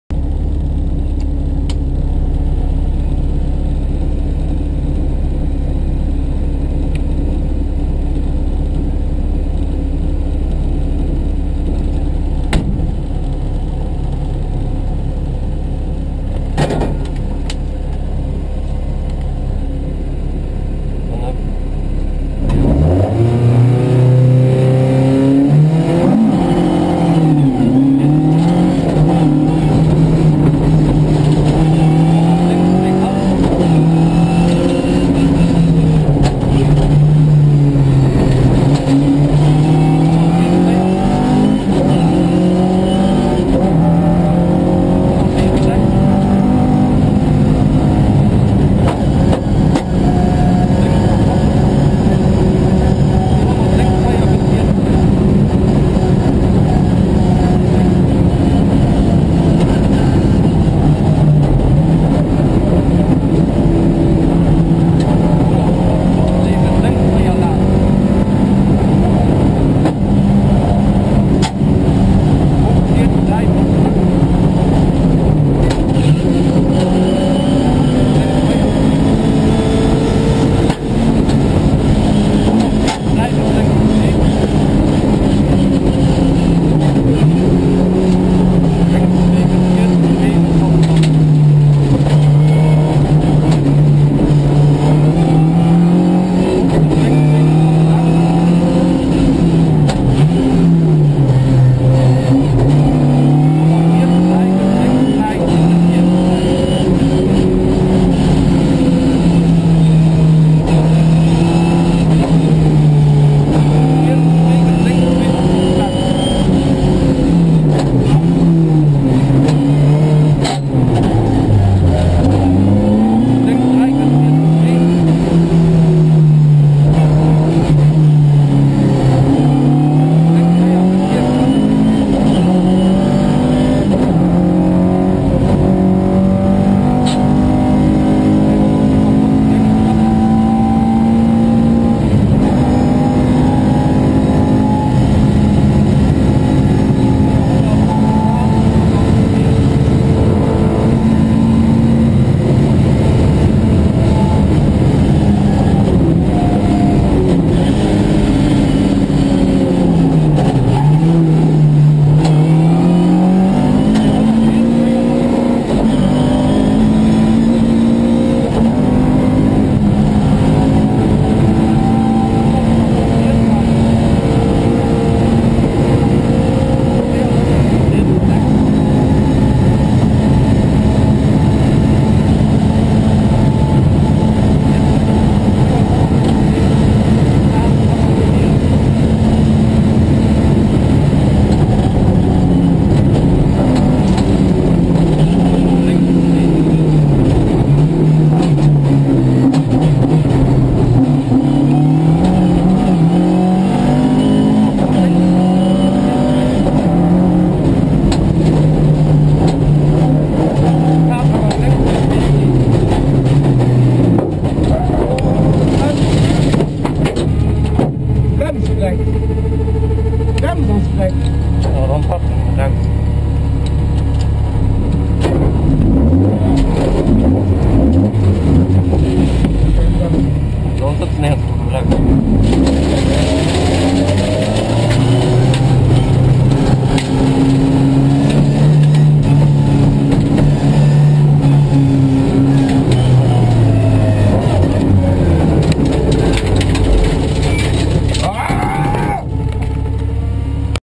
Rallye Claudy Desoil 2018 ES 5 abandon
Equipage SCHLOESSER - DUTON

Ford Escort MKII